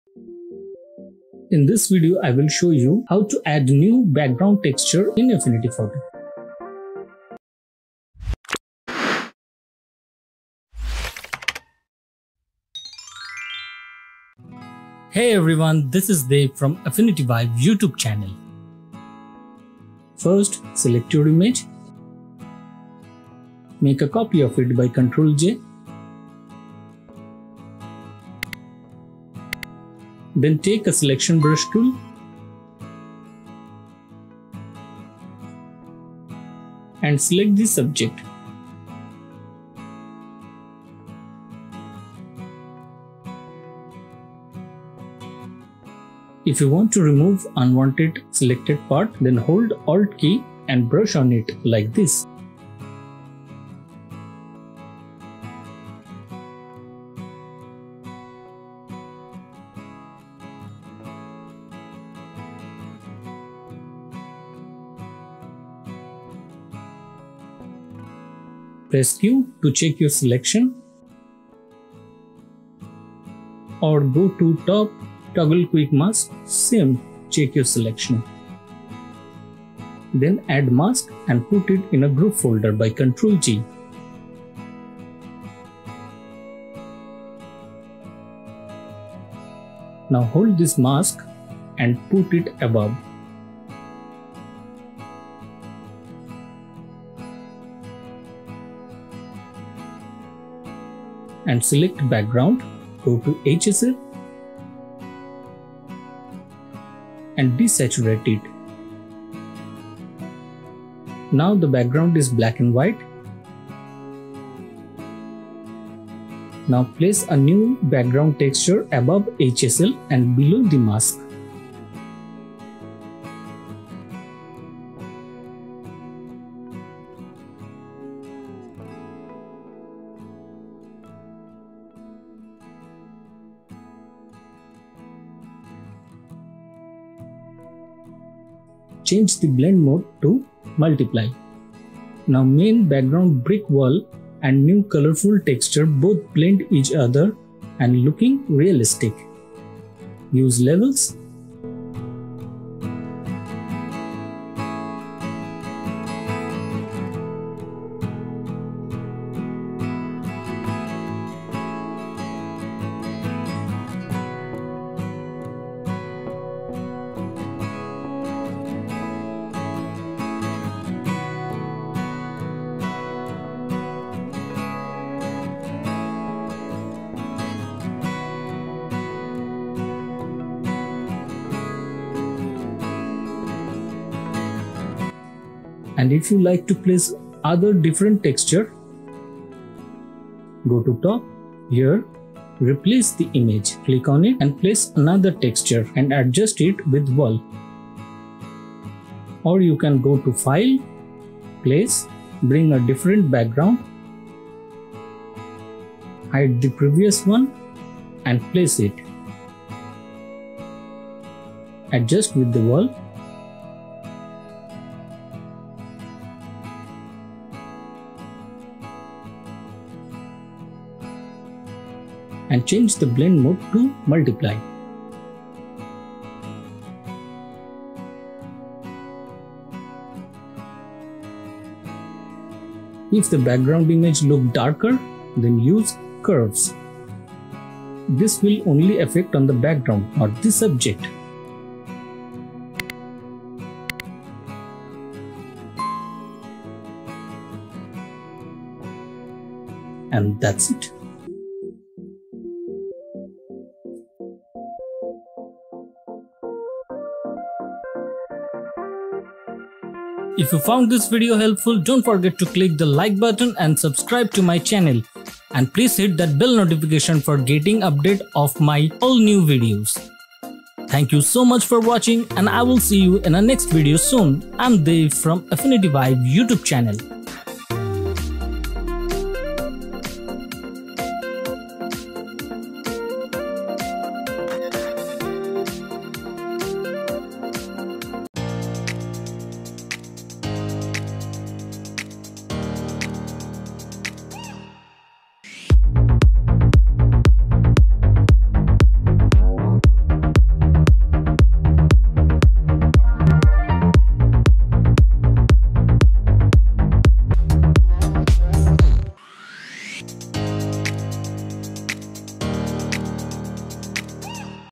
How To Add Background Texture in Affinity Photo | Affinity Photo Tutorial
Hey everyone,
I'm Dav and In this video, you’re going to know exactly How To Add Background Texture in Affinity Photo. This is Affinity Photo Tutorial on Add Background Texture

👉 Pls watch his video till the end... I appreciate your LIKE / COMMENT/ SHARE !! 🙂

You Can Also Check Out There Videos